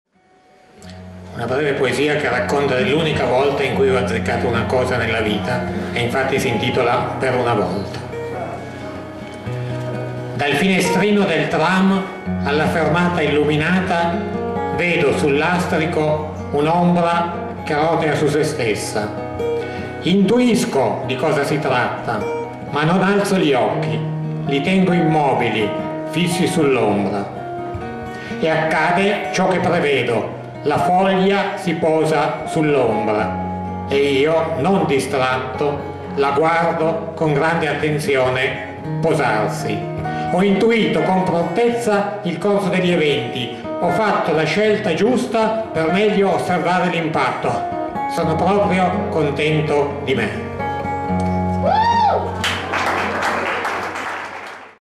Per una volta
"Per una volta": poesia inedita di Carlo Molinaro al Concertino dal balconcino del 26 novembre 2017. Accompagnato da Simone Dani.